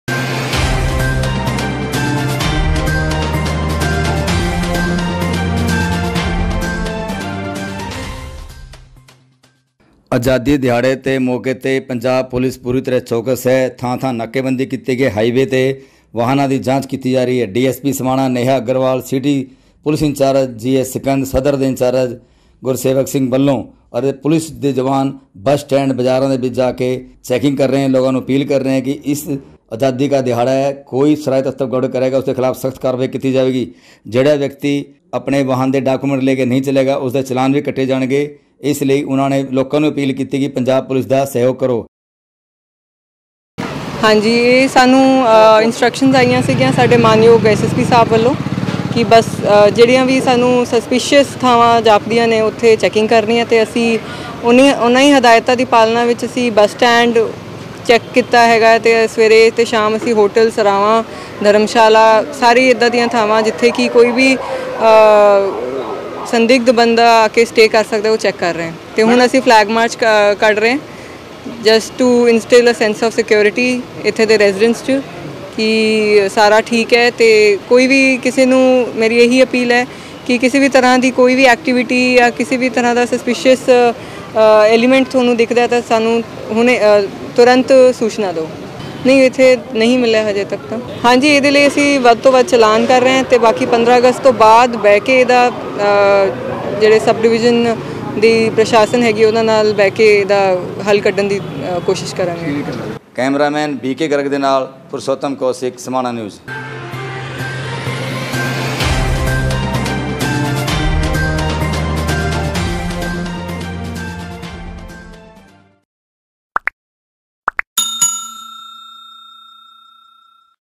आजादी दिवस के उपलक्ष में पंजाब पुलिस ने चेकिंग अभियान किया शुरू, नाकेबंदी कर रही है हर वाहन की जांच
आजादी दिवस के उपलक्ष में पंजाब पुलिस पूरी तरह चौकस है कोई शरारती तत्व इस आजादी दिवस के मौके पर प्रार्थना करें उसके लिए पंजाब पुलिस ने 24 घंटे पेट्रोलिंग शुरू कर दी है और हाईवे सड़के हैं जो  उन पर नाकेबंदी करके वाहनों की जांच की जा रही है समाना- पातड़ा सड़क पर पटियाला जाने वाली सड़क  पर सदर पुलिस थाना ने नाकेबंदी  की हुई है और रात के अंदर भी यह चेकिंग चल रही है इसके बारे में जानकारी देते डीएसपी समाना नेहा अग्रवाल , सिटी पुलिस के इंचार्ज  जी एस  सिकंद , समाना सदर के  इंचार्ज  गुरसेवक सिंह,पुलिस अधिकारी  आजादी का उत्सव है मुख्यमंत्री पंजाब पटियाला में आजादी दिवस के मौके पर ध्वजारोहण करेंगे इसलिए विशेष हिदायतें पुलिस को  दी गई हैं  किसी तत्व को गड़बड़ करने की इजाजत नहीं दी जाएगी  उन्होंने लोगों से अपील की कि पुलिस का सहयोग करें
Beyt-1  नेहा अग्रवाल डीएसपी समाना
                    पुरुषोत्तम कौशिक समानाl